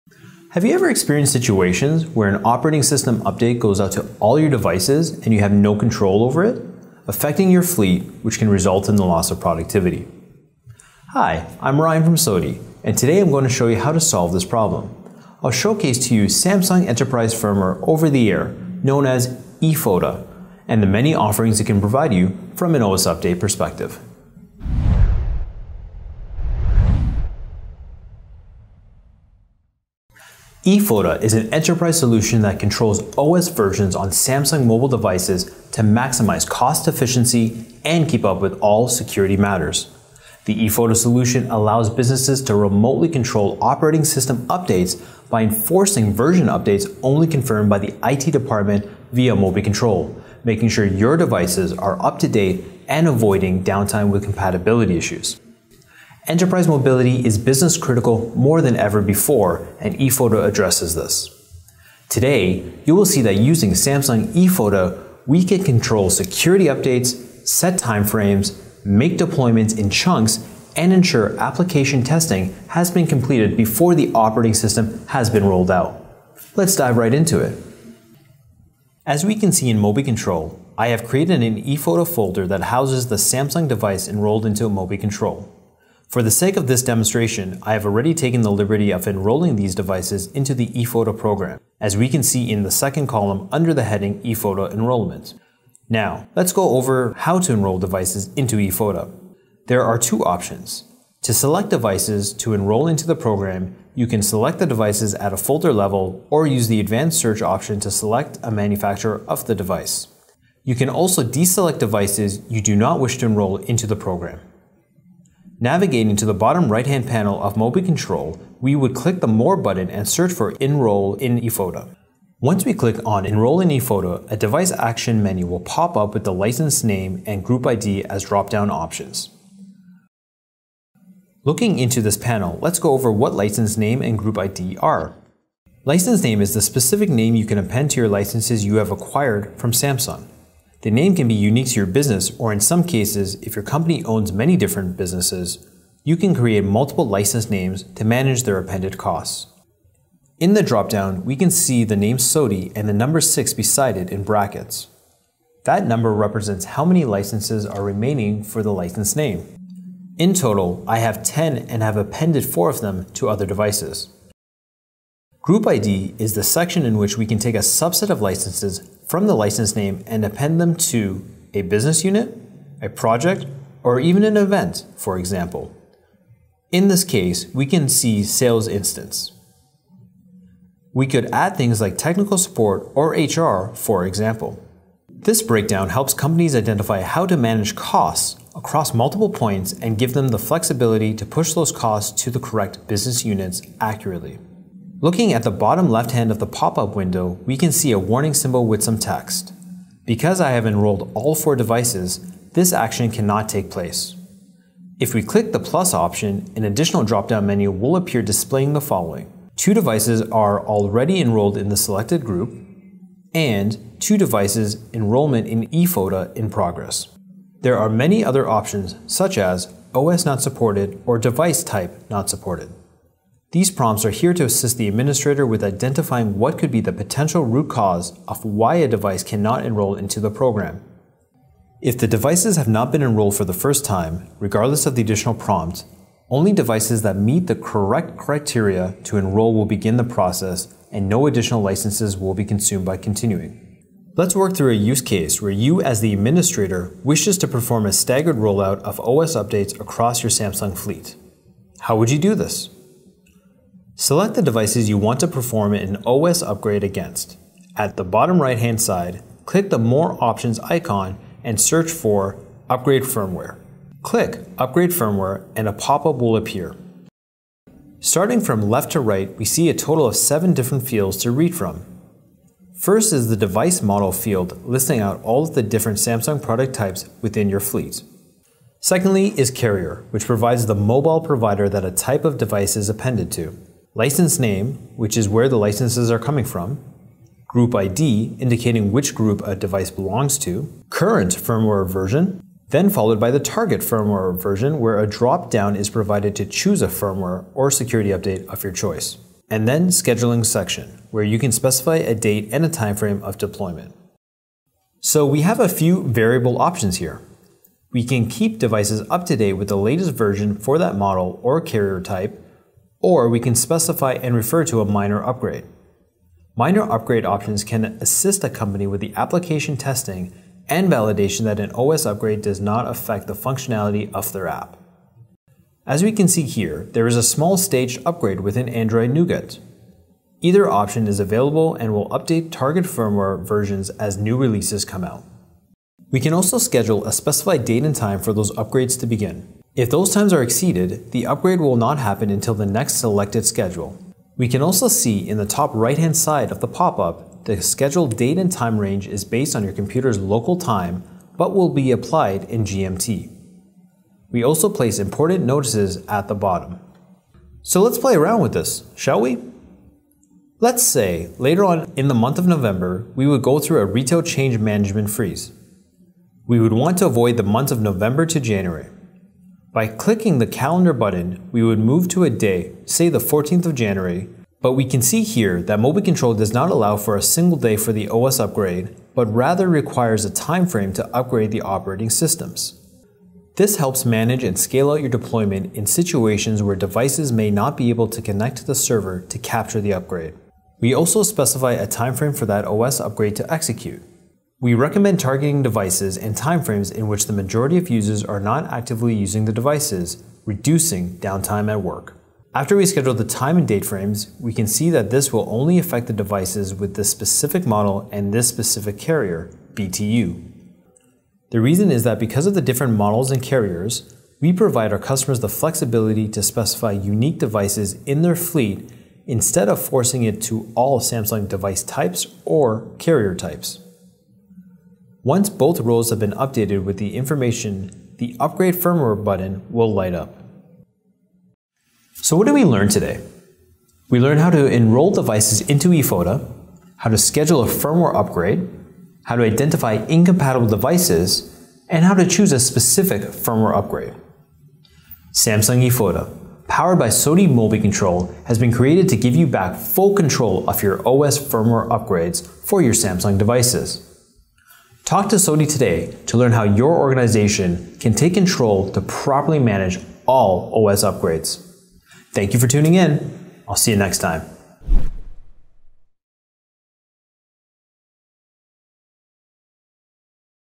SOTI MobiControl and Samsung’s E-FOTA Increases Security, Productivity and Maximizes Cost Efficiency
Join Ryan from SOTI, as he demonstrates how SOTI MobiControl works with the Samsung Enterprise FOTA (E-FOTA) solution to help organizations take back control of their OS versions on Samsung mobile devices with easy to use remote technology to properly manage OS upgrades.